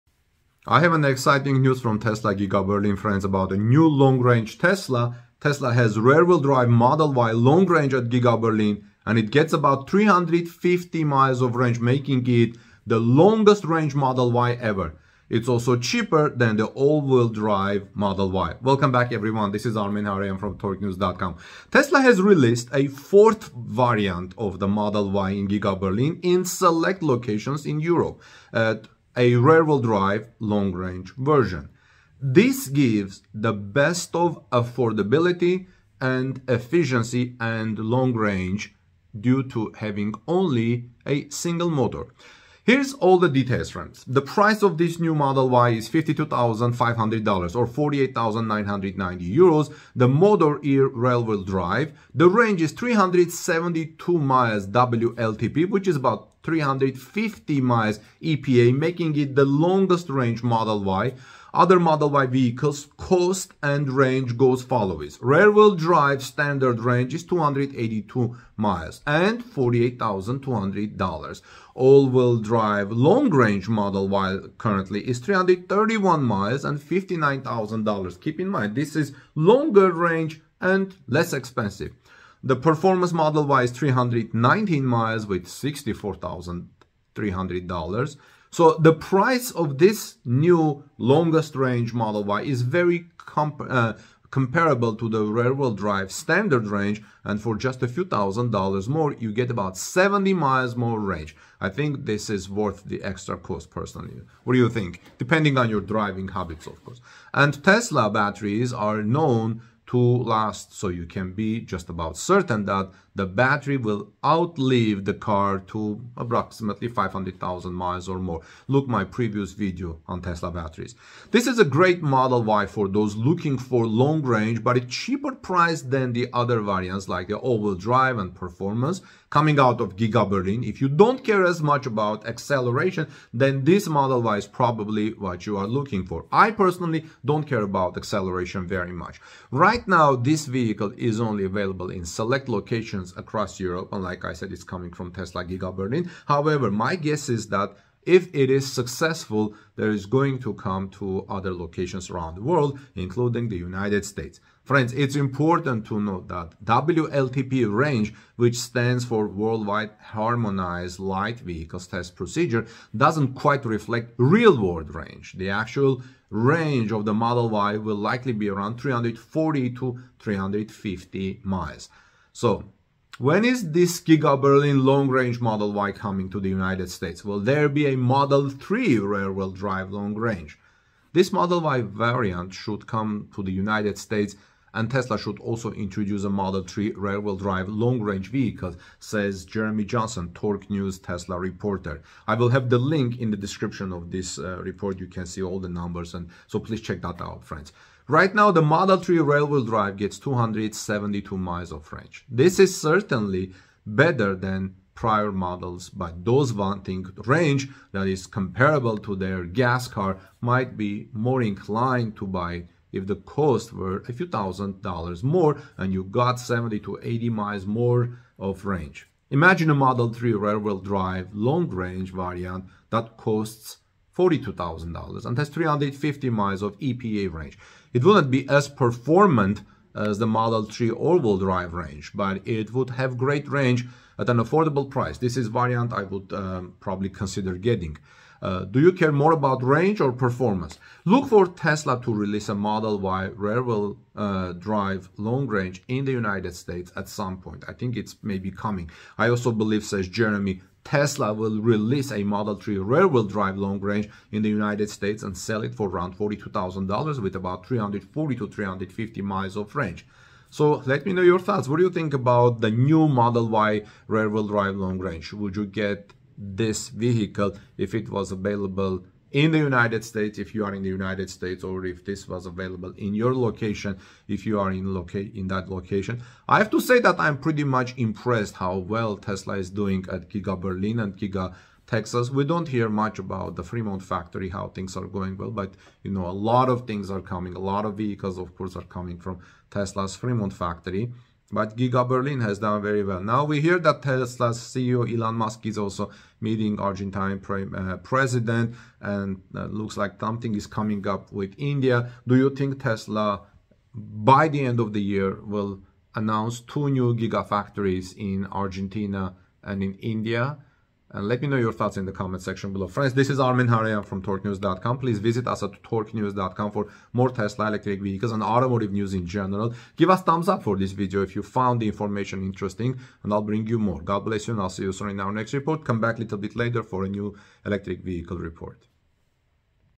Tesla Giga Berlin Presents The Longest Range Model Y So Far With 350 Miles of Range
Tesla has a RWD Model Y long range at Giga Berlin - and it gets about 350 miles of range, making it the longest range Model Y ever. It's also cheaper than the AWD Model Y.

This is a great Model Y for those looking for long range, but a cheaper price than the other variants like the AWD and performance. If you don't care as much about acceleration, then this Model Y is probably what you are looking for.

Here are some of the specs of Tesla Giga Berlin's longest range Model Y so far.

Price: $52,500 (or 48,990 Euros)
Motor: RWD (rear-wheel-drive)
Range: 372 (WLTP) or about 350 EPA
Other Model Y Vehicles Cost and Range:
RWD Standard Range (282 miles): $48,200
AWD Long Range (331 miles): $59,000
Performance (319 miles): $64,300